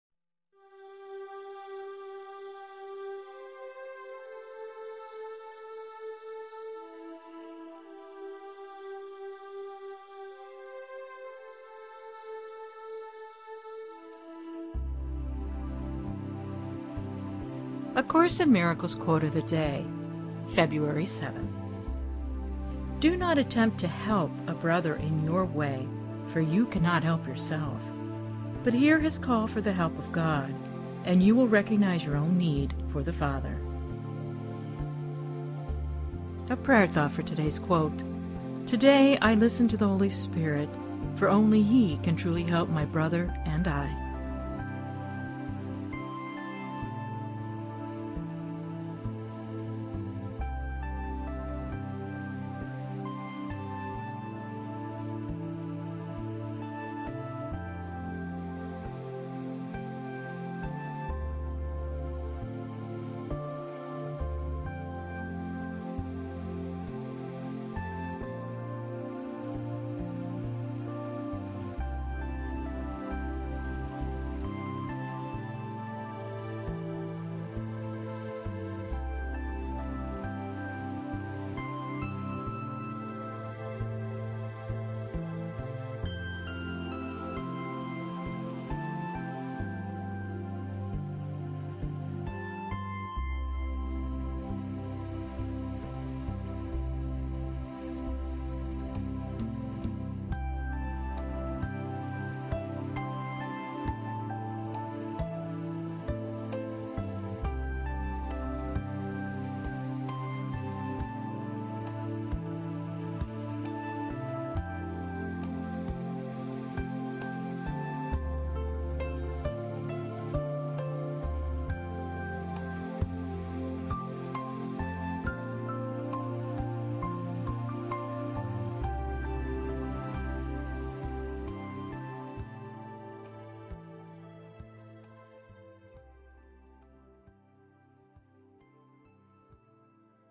A Course in Miracles Quote for February 7th
"Do not attempt to "help" a brother in your way, for you cannot help yourself. But hear his call for the Help of God, and you will recognize your own need for the Father." T-12.I.6:10-11